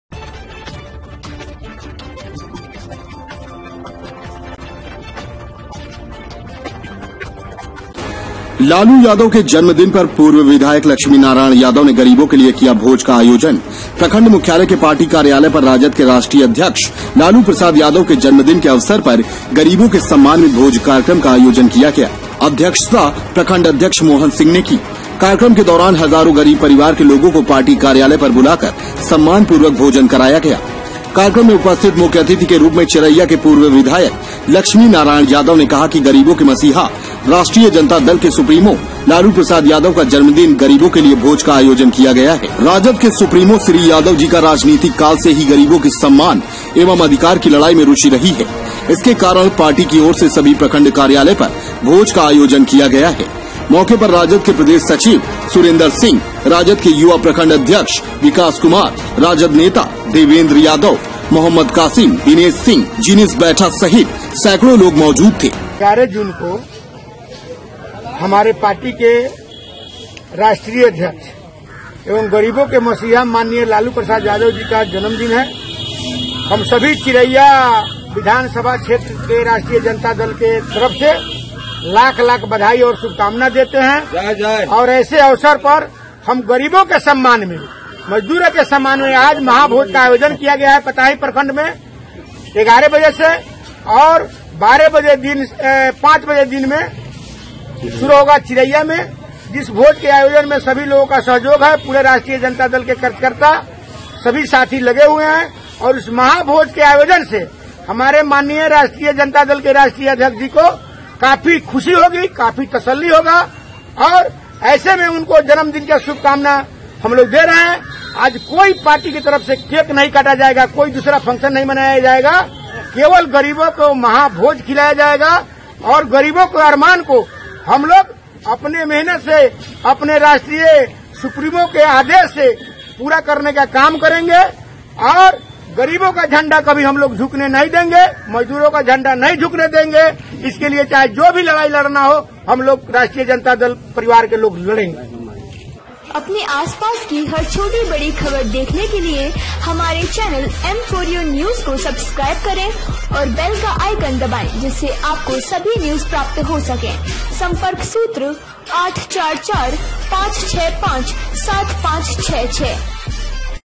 लालू यादव के जन्मदिन पर पूर्व विधायक लक्ष्मी नारायण यादव ने गरीबों के लिए किया भोज का आयोजन
बिहार न्यूज (M4UNEWS)